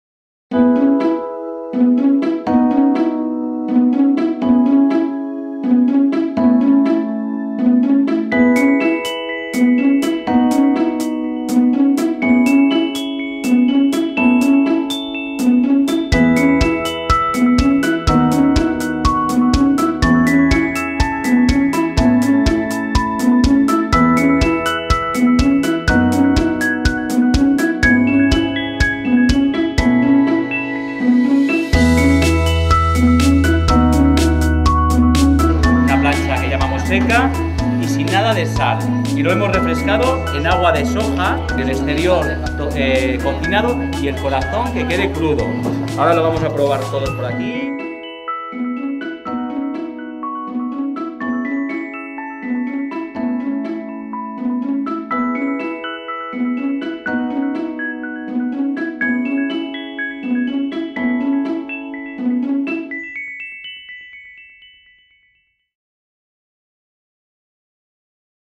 Ronqueo y degustación del atún rojo de almadraba
El atún rojo de almadraba es un bocado único. Alimerka lo acerca a sus clientes y para que lo conozcan, organizó un ronqueo y una degustación de la mano de Sergio Rama.